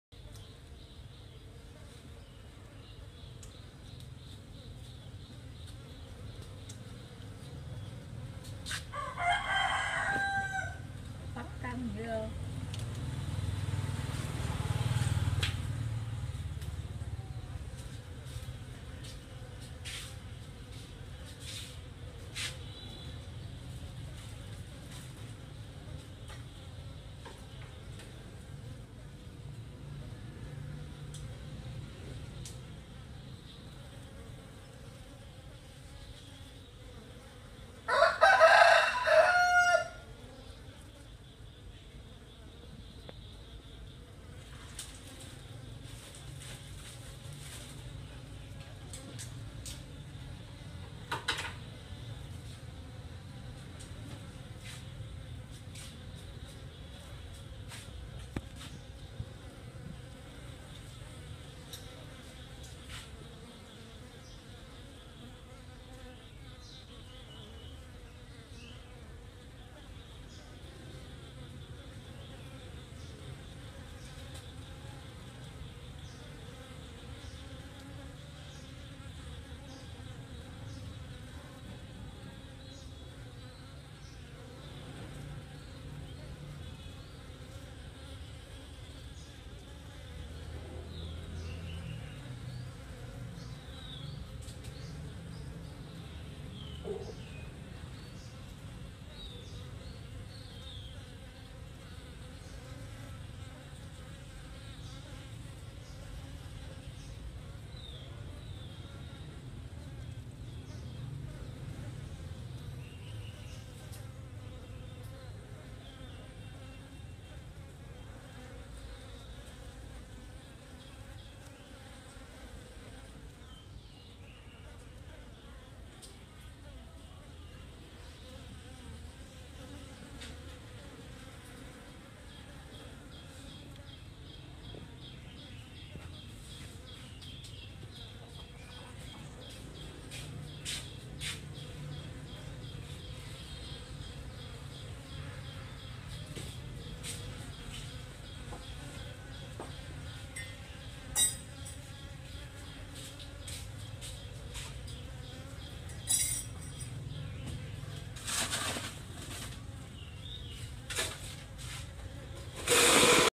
Con ong (apis cerena) đem cuộc phấn hoa về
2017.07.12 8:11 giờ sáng